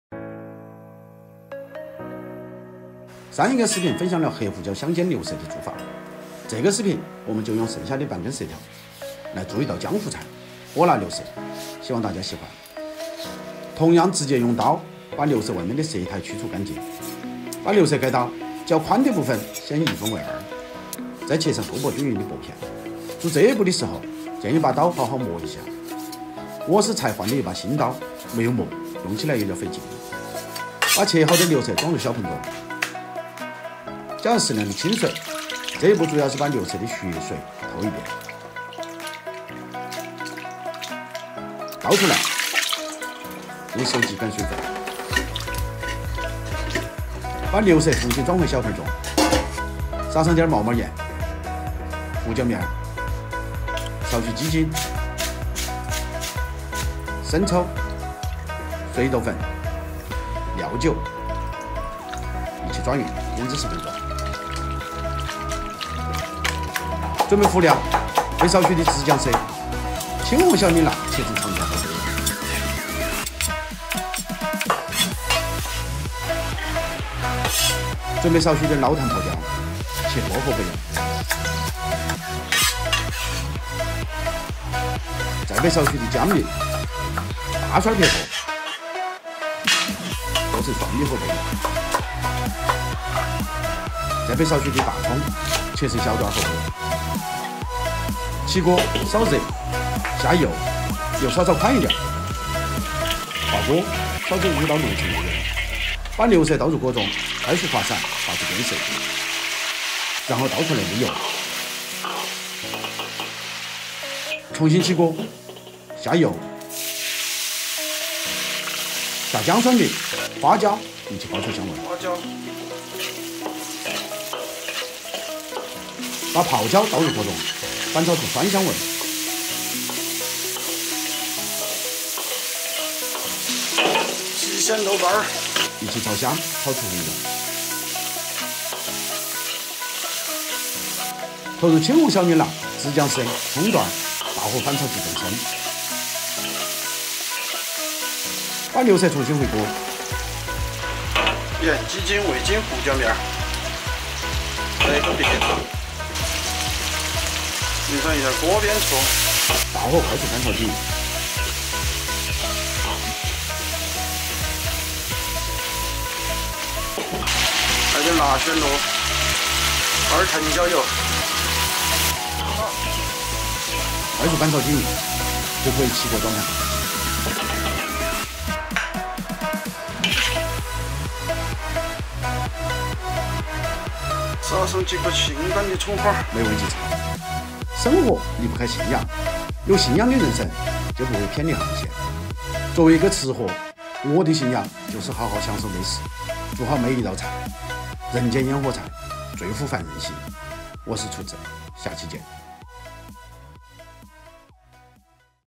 做牛舌還是喜歡重慶的做法，江湖菜，大盤大碗，麻辣勁爆，巴适【厨子说菜】
上次給大家分享了一道黑胡椒牛舌的做法，今天就用剩下的半根牛舌給大家做一道重慶美食。人間煙火菜，最撫凡人心。
每天幾分鐘，輕鬆學會做菜

韭菜雞蛋究竟是先炒韭菜還是先炒蛋，廚子給你答案。正宗尖椒鱸魚，豬頭肉……你喜歡的好吃的，這裡都有。